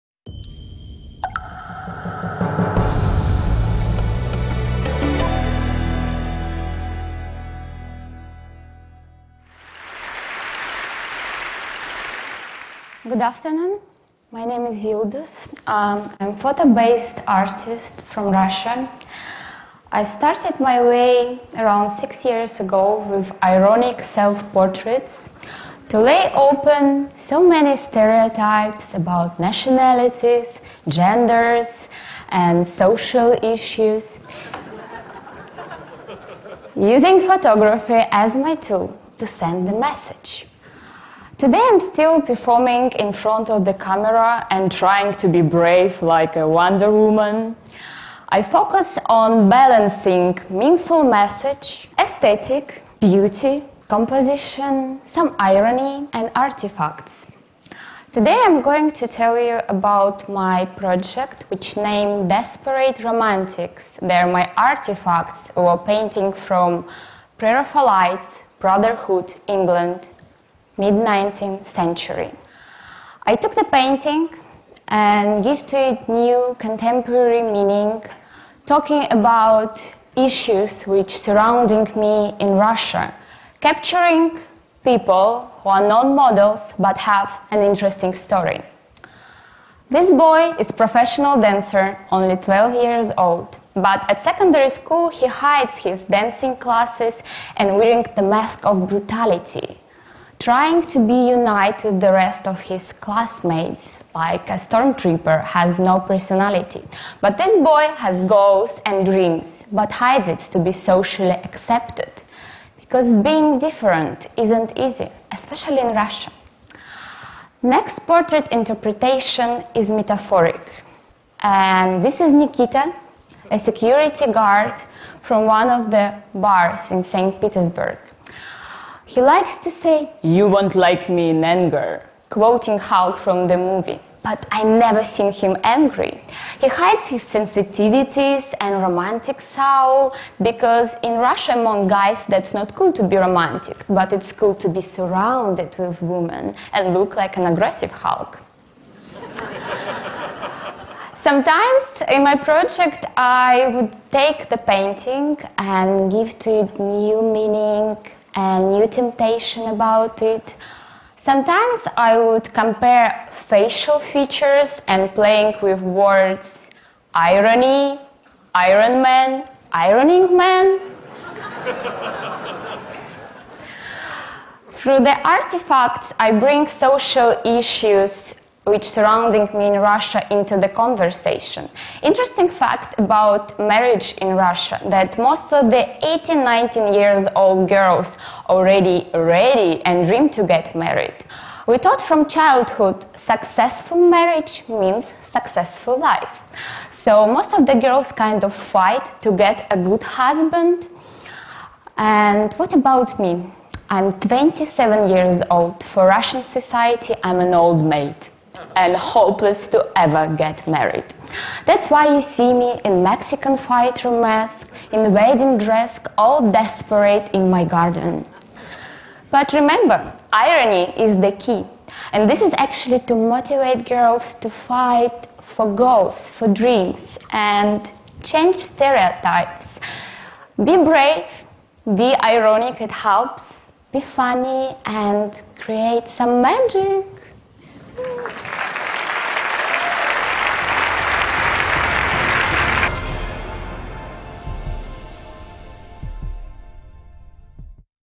Ironic photos that turn stereotypes upside down 顛覆刻板印象的諷刺照片
藝術家優都思．巴克實茲娜用照片開母國俄羅斯社會規範的玩笑。簡短又有趣的演講帶你一瞥俄羅斯青年文化，並提醒你別把事情看得太認真。